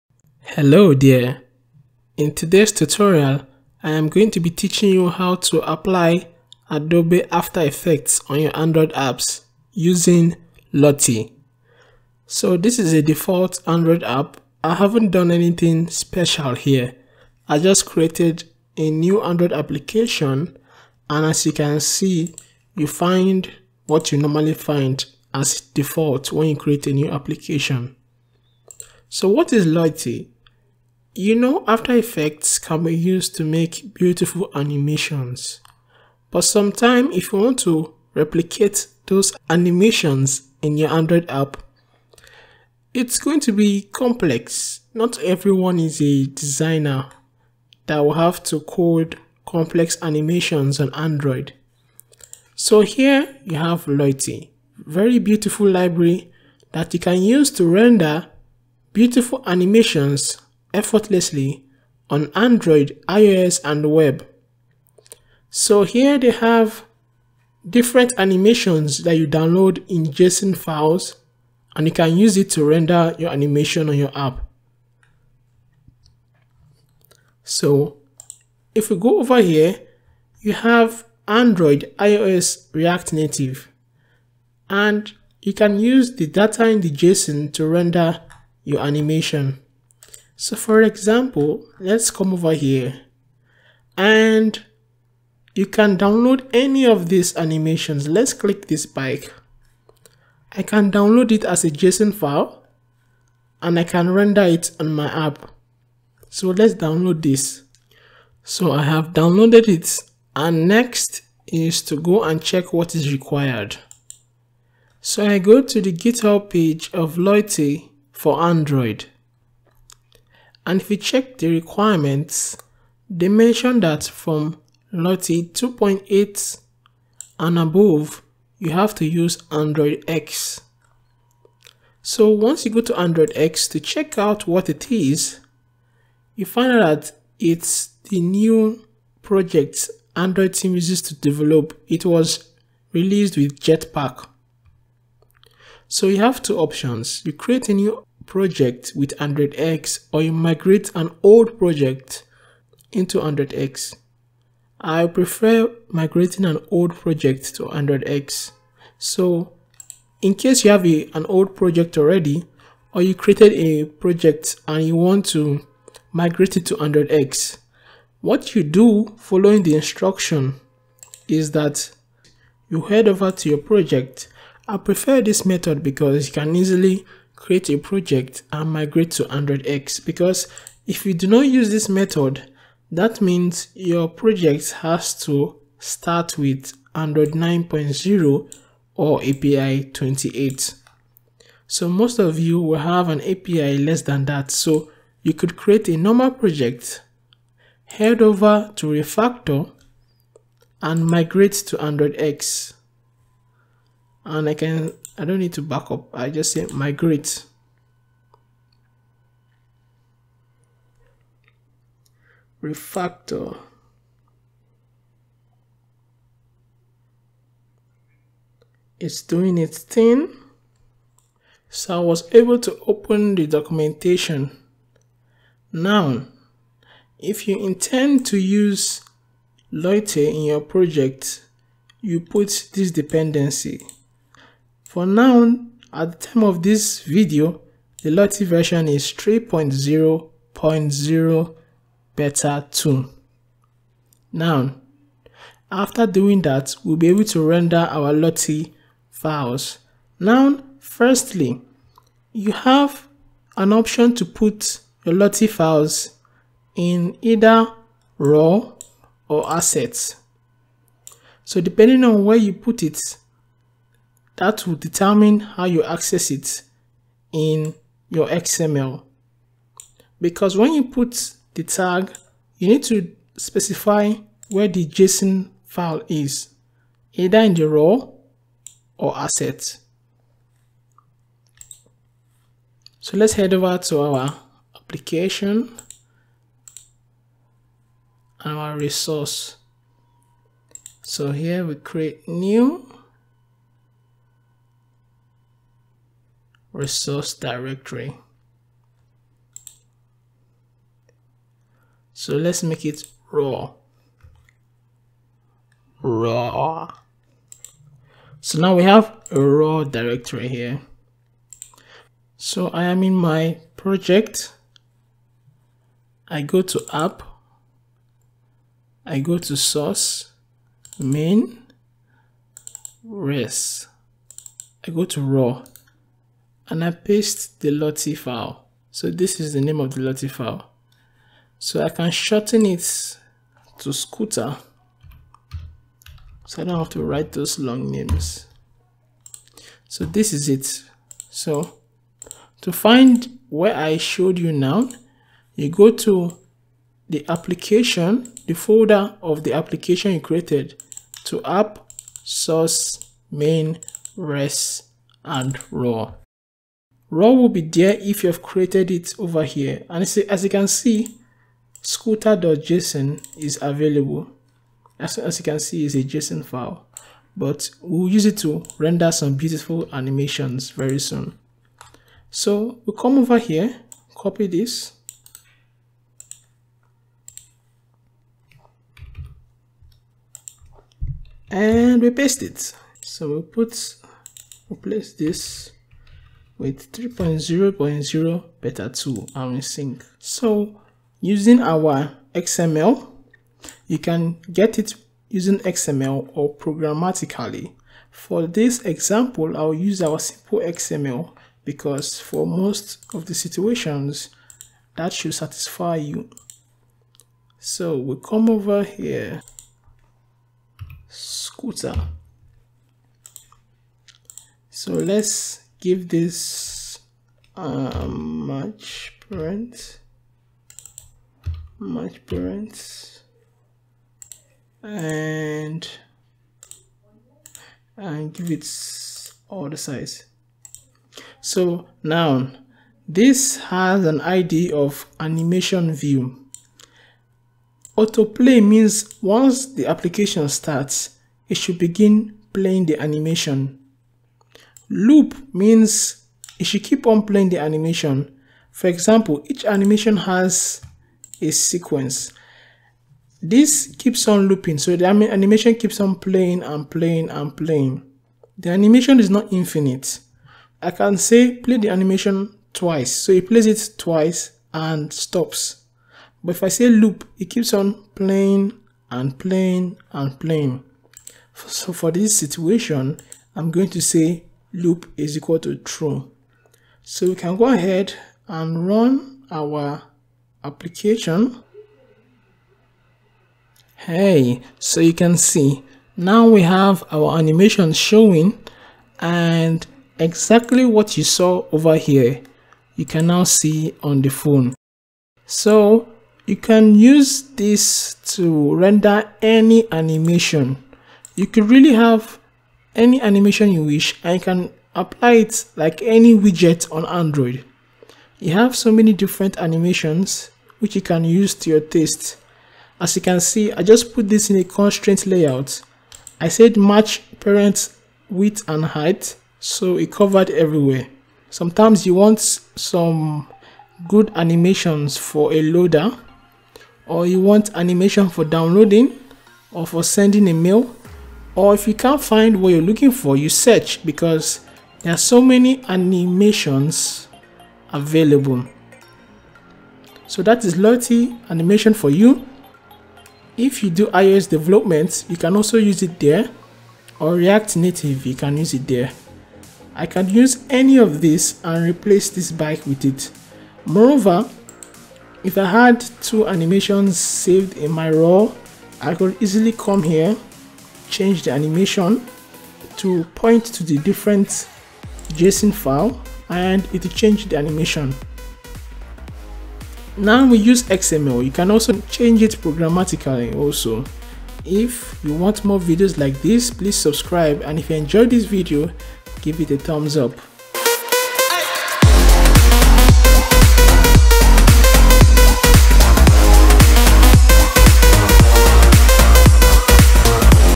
Rendering Cool Android Animations Using Lottie
In this tutorial, you would learn how to display animations in Android using in a splash screen or in the app activity using Lottie. Lottie is an iOS, Android, and React Native library that renders After Effects animations in real time, allowing apps to use animations as easily as they use static images. For the first time, designers can create and ship beautiful animations without an engineer painstakingly recreating it by hand. If you are developing on Android with either Kotlin or Java you can still use Lottie.